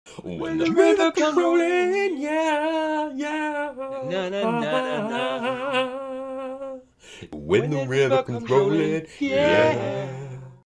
vocal test
a mic test using Yeti USB cartiod pickup v pc